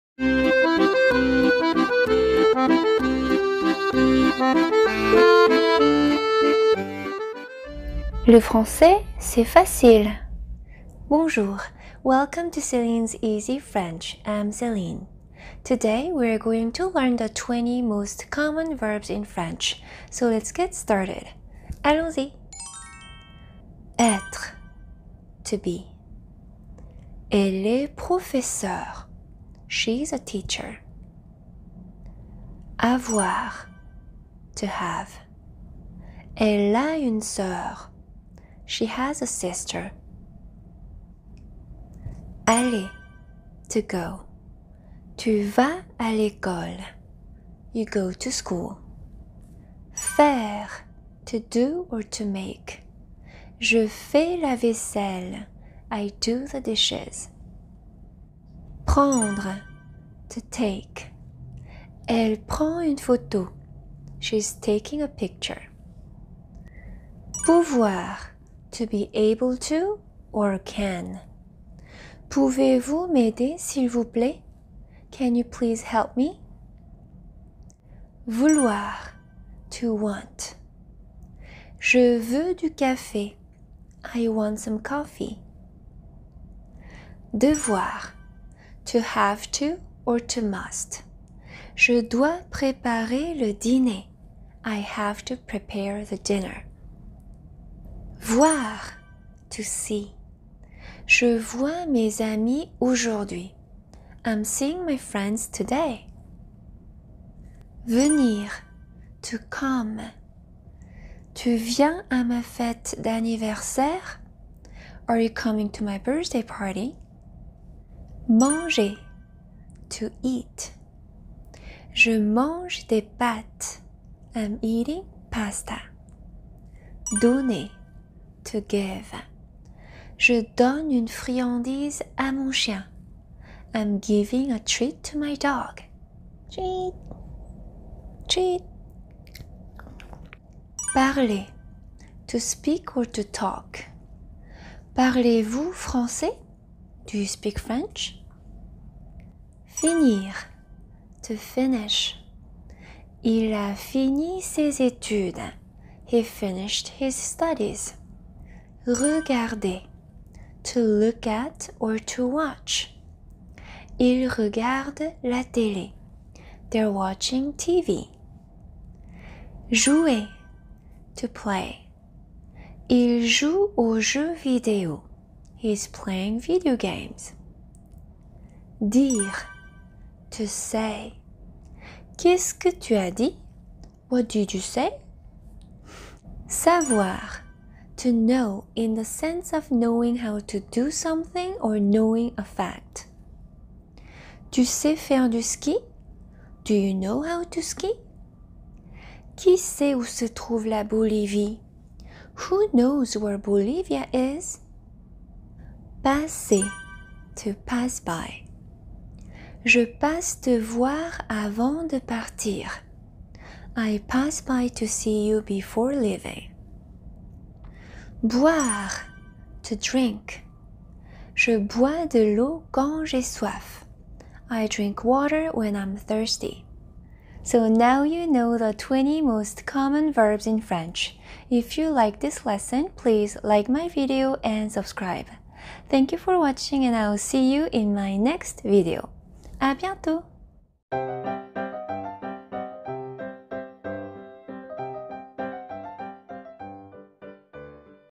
🇨🇵TOP 20 FRENCH VERBS - THE MOST COMMON VERBS IN FRENCH (Learn French Lesson 13)🇨🇵
Bonjour mes chers amis!

In this lesson, you will learn the most common verbs in French! It is fast, easy and fun!

Merci et bises 💕